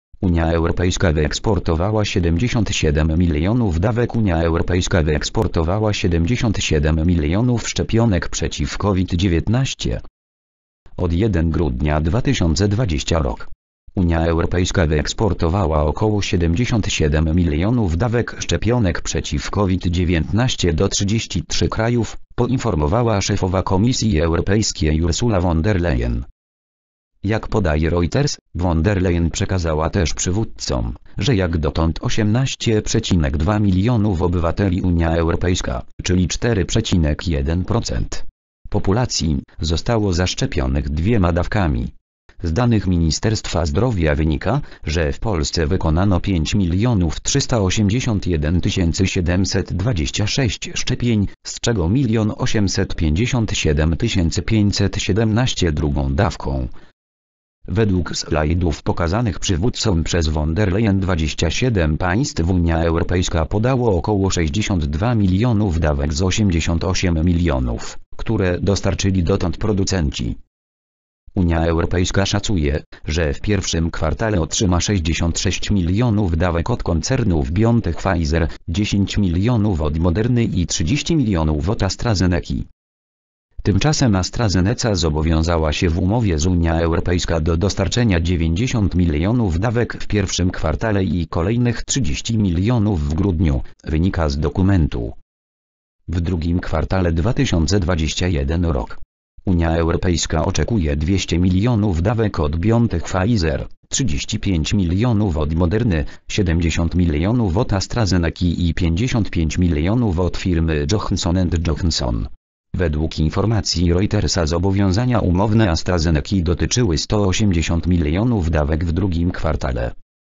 UE wyeksportowała 77 mln dawek.
UE wyeksportowała 77 milionów szczepionek przeciw COVID-19.

  (fot. PAP/EPA)